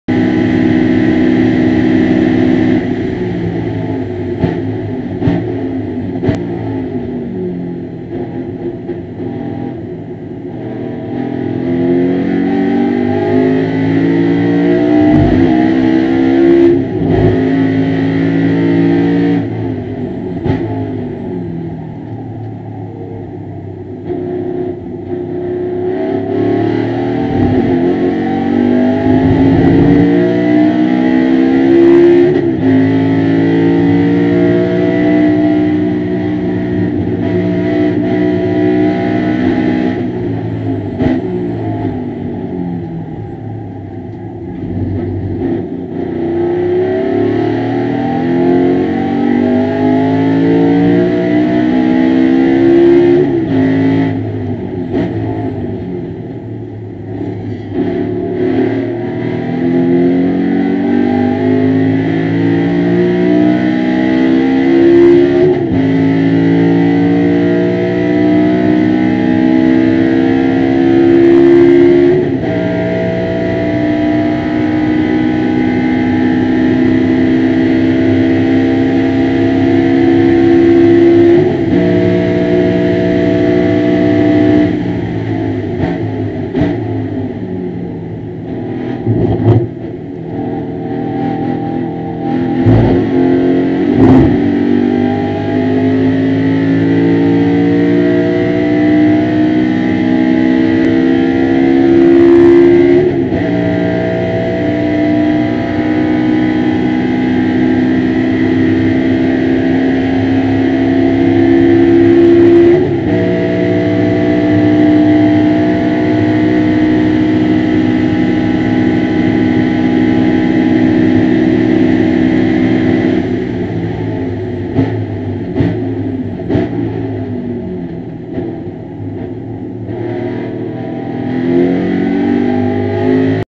Daytona Saturday Test
Testing the BMW E92 Gs M3 during the conitinental ROAR before the 24 at Daytona International Speedway